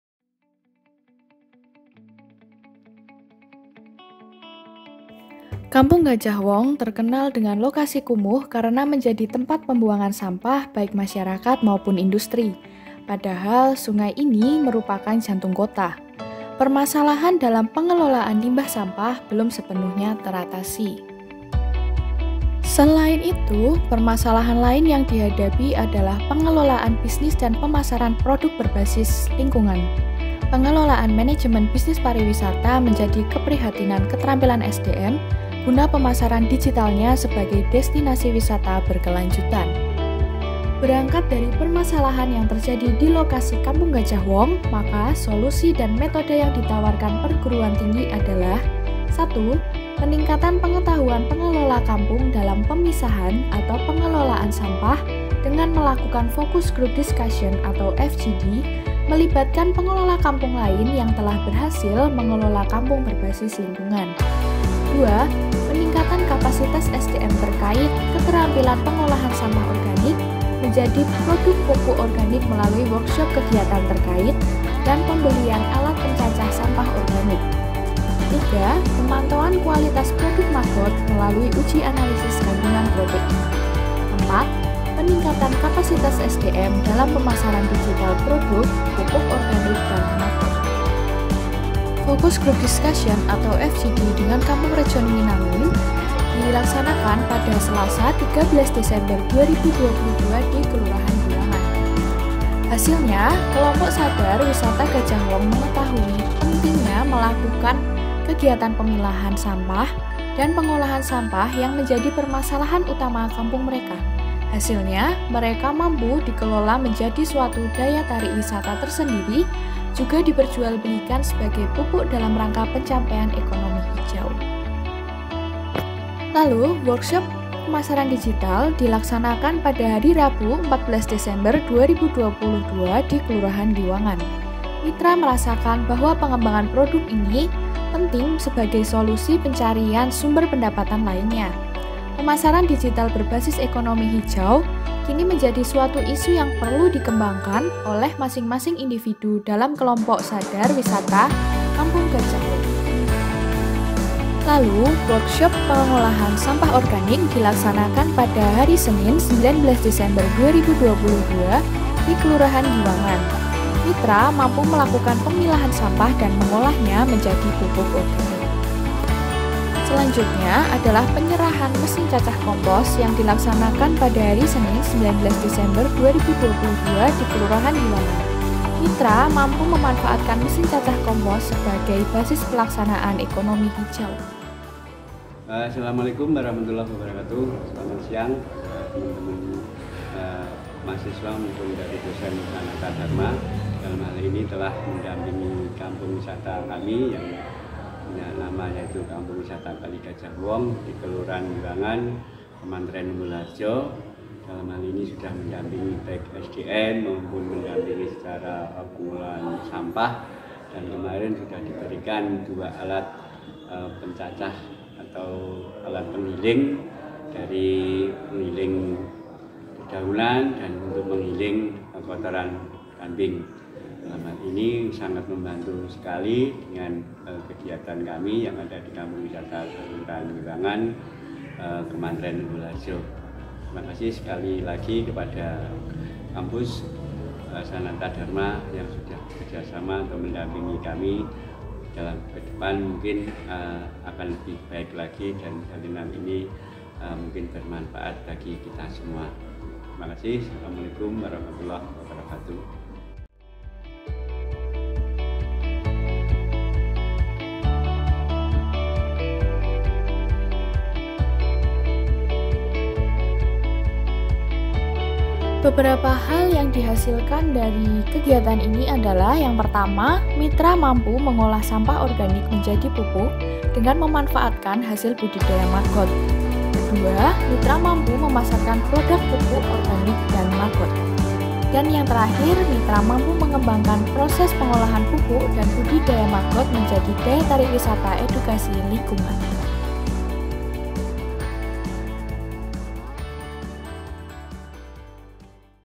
Kegiatan Kemandirian Masyarakat - Kelompok Sadar Wisata Kampung Gajah Wong
Peningkatan Kapasitas Pengelola Kampung Gajah Wong dalam Pengembangan Produk dan Pemasaran berbasis Ekonomi Hijau.